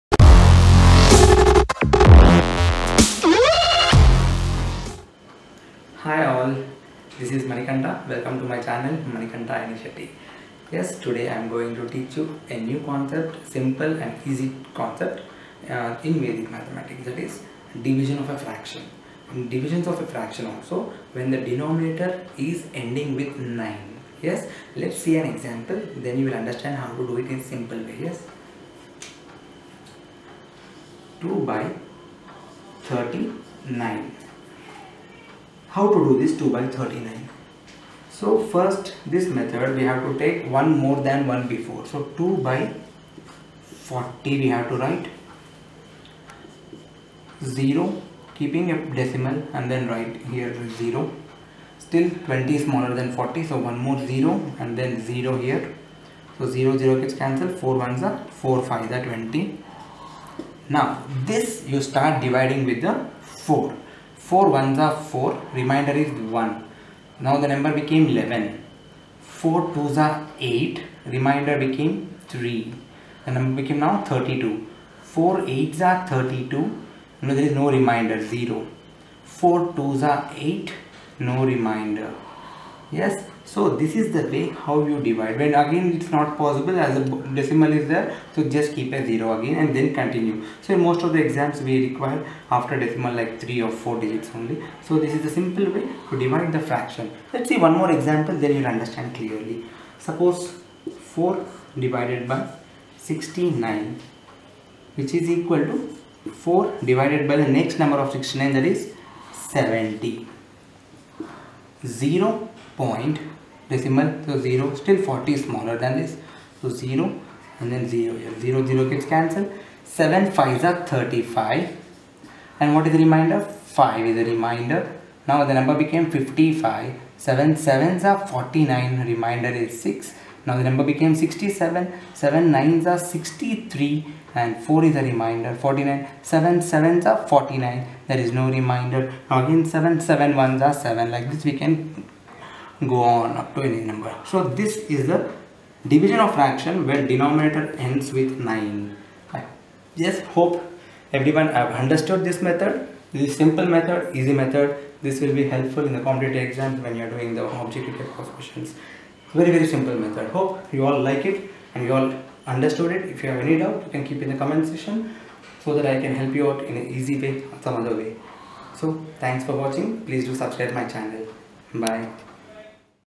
Fractions Division Denominator ending with 9 | Vedic Math Technique | Solve in one step
Solve the fractions division, denominator ending with nine. Solving in just one step.
By using Vedic Math method - one more than one before.